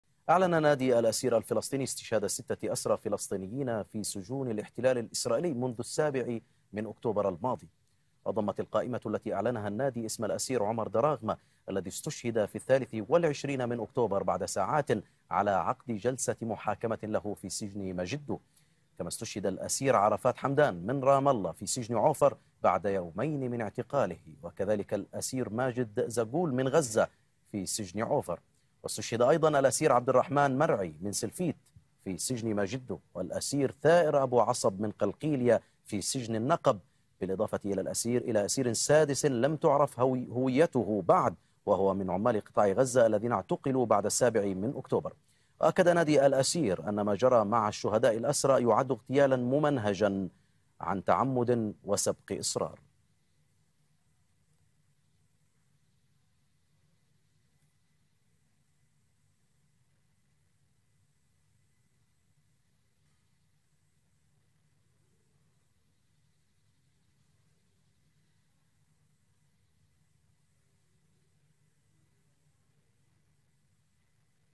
ستة أسرى شهداء في سجون الاحتلال بعد السابع من أكتوبر.. إليكم التفاصيل
أعلن نادي الأسير الفلسطيني استشهاد 6 أسرى فلسطينيين في سجون الاحتلال الإسرائيلي منذ 7 أكتوبر الماضي وكشف عن هوياتهم وتواريخ وفاتهم..